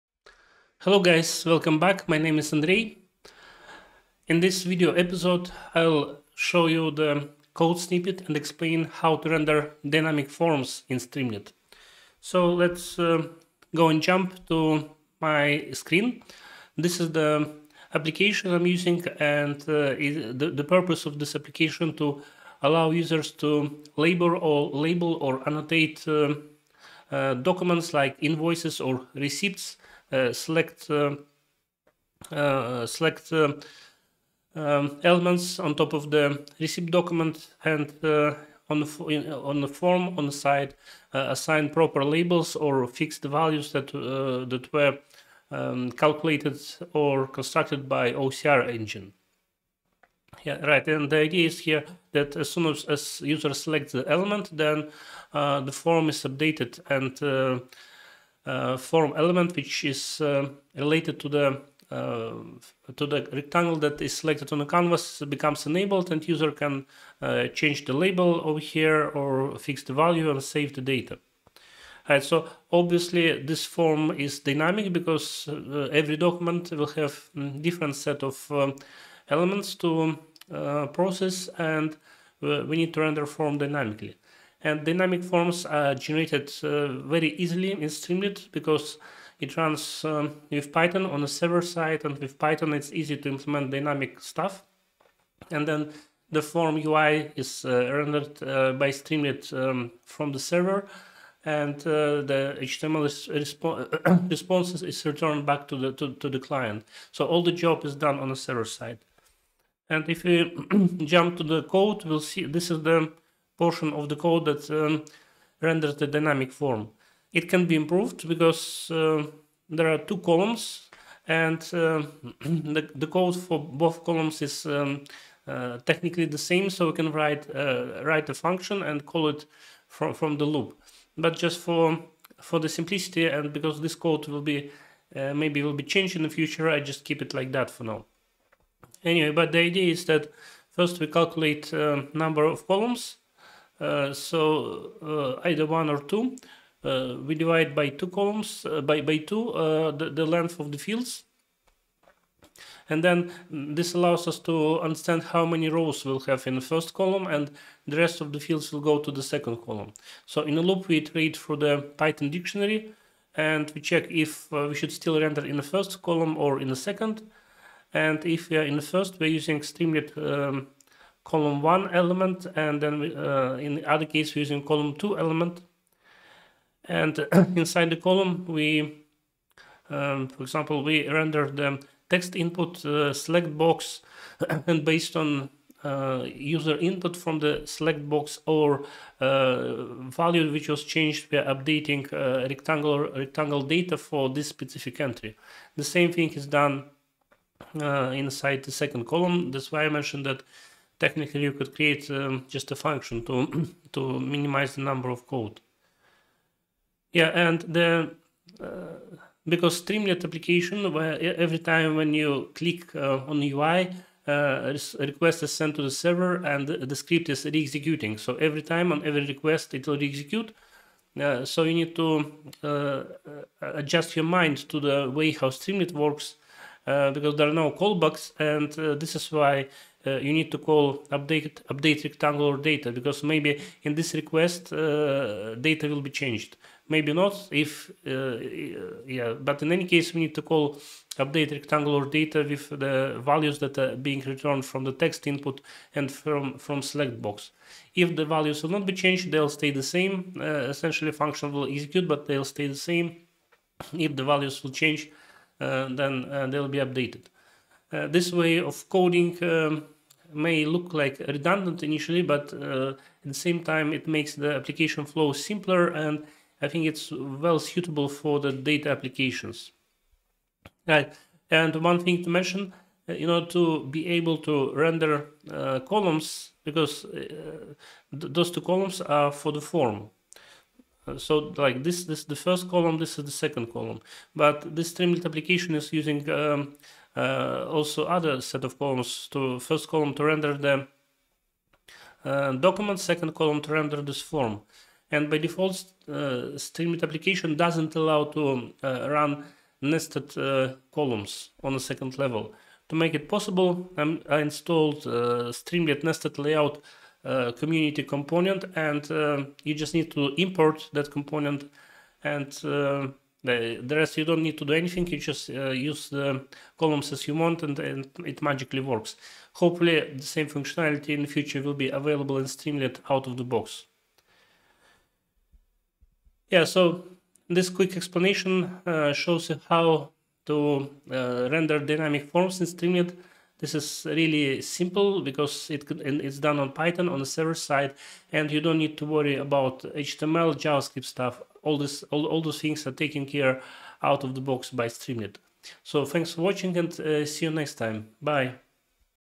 Building Streamlit/Python Dynamic Forms
Streamlit is great to build Web UIs for data apps. Dynamic UIs are common requirement for data apps. Data structures usually dynamic, set of attributes is changing and this requires dynamic UI. I explain how to generate dynamic forms with Streamlit. It works very smooth.

0:00 Introduction
0:13 Example
1:51 Dynamic Form Code
4:42 Example
4:57 Column Nesting
5:27 Summary